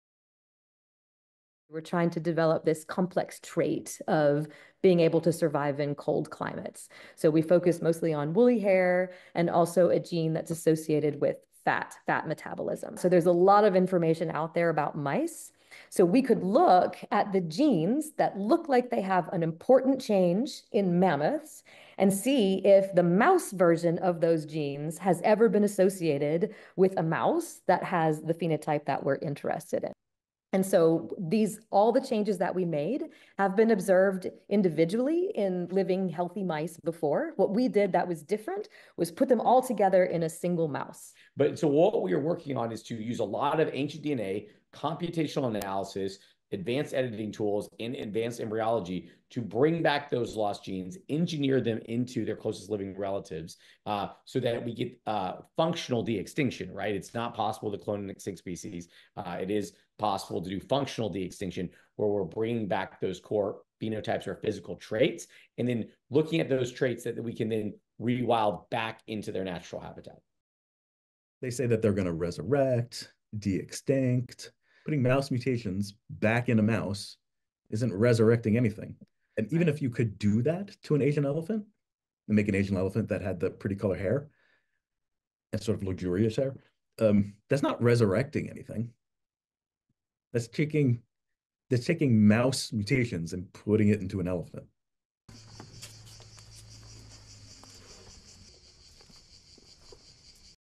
Scientists genetically engineer “Colossal woolly mouse”
Scientists at a biotech company are genetically engineering living animals with qualities to resemble extinct species like the woolly mammoth. Colossal Biosciences announced Tuesday that it has simultaneously edited seven genes in mice embryos to create mice with long, thick, woolly hair.

Scientists nicknamed them the “Colossal woolly mouse.” The results posted online have not yet been published in a journal or vetted by independent scientists. Outside scientists have mixed views on whether this strategy will be helpful for conservation.

Video courtesy Colossal Biosciences via AP